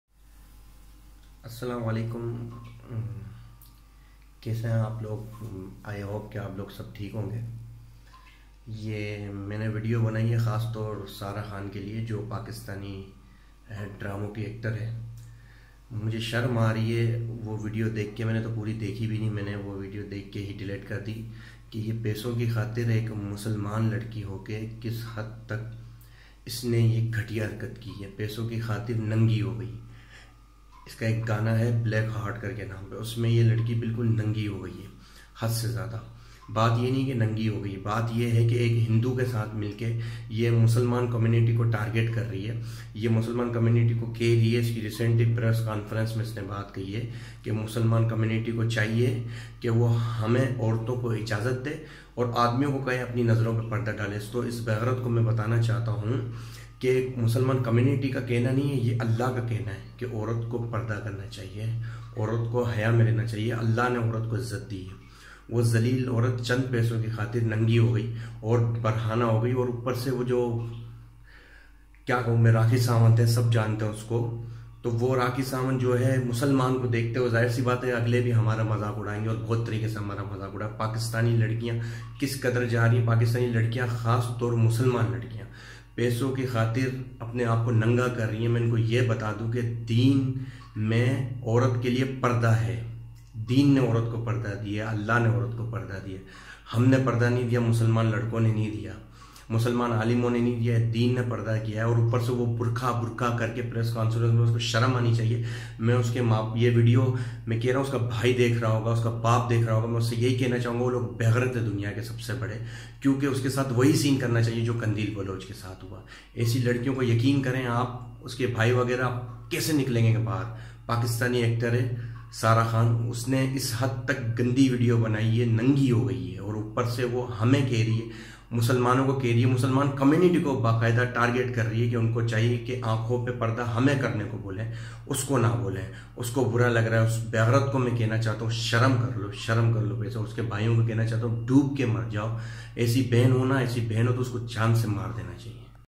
:Sara khan sharam Karo khud ko Muslim kehti ho:
Chand peso ki khatir khud ko Nanga Kar Dena yeh Muslim larki ki shan nahi our sharam Ani chye tum jc jitni larkiyan hen Un sub ko kuch haya hoti ek hindu larki k Sam ne Muslim comenity ko target karts howe